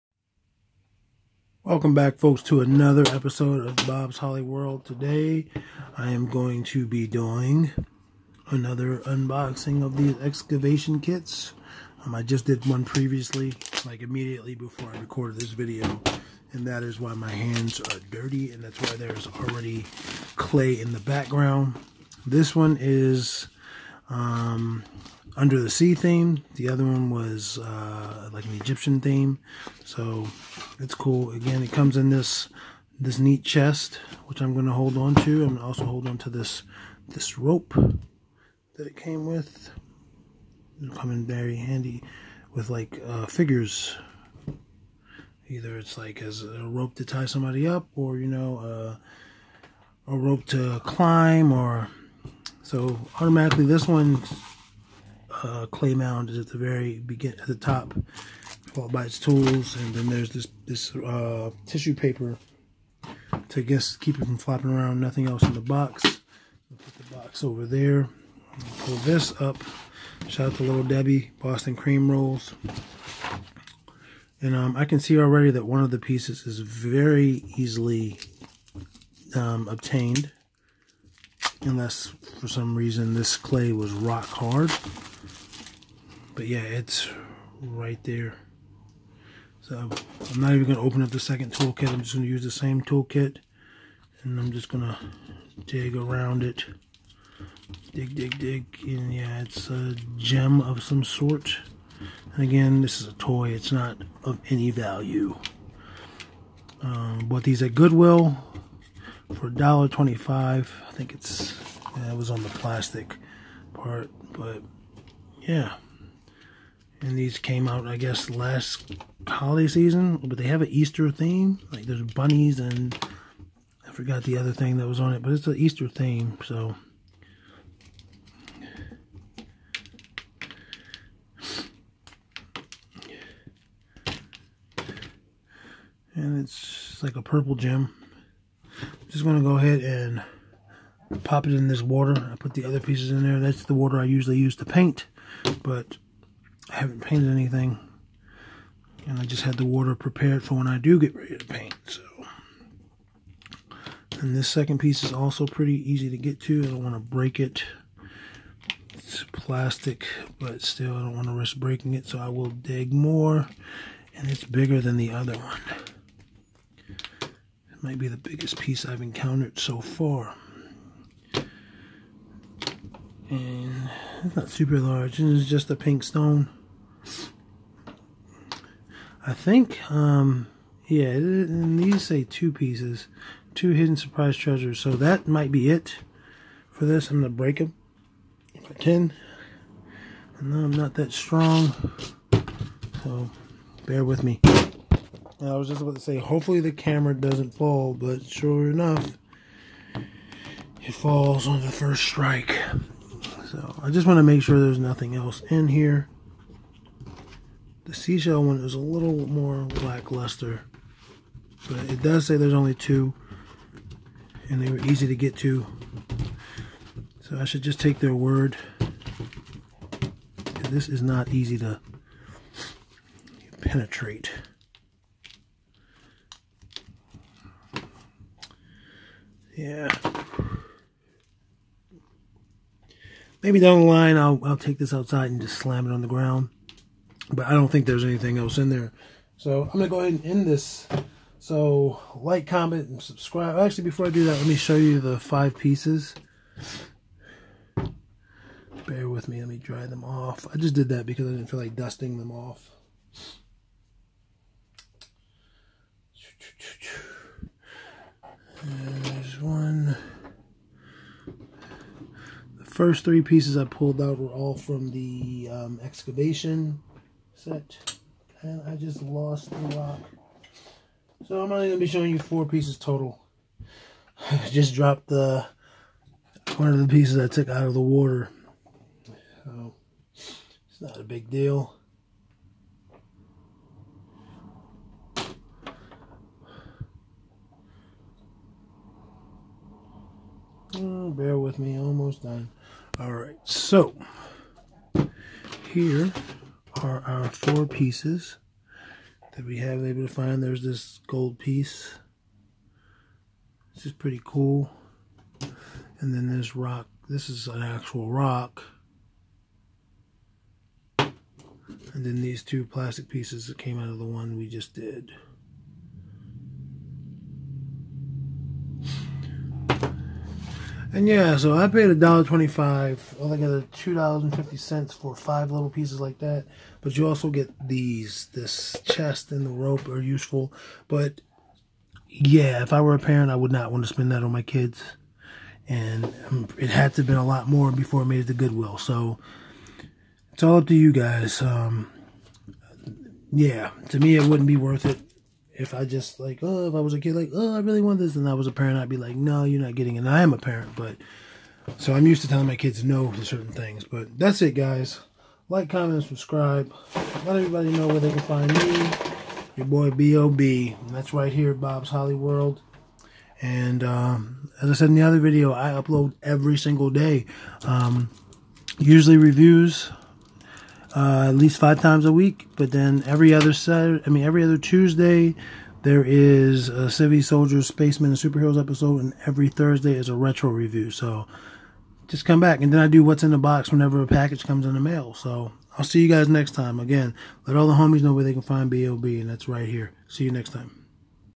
Under the Sea Excavation Kit
Another kit from Goodwill, and I'm even less impressed with this one. Again, the packaging is the most valuable part to me.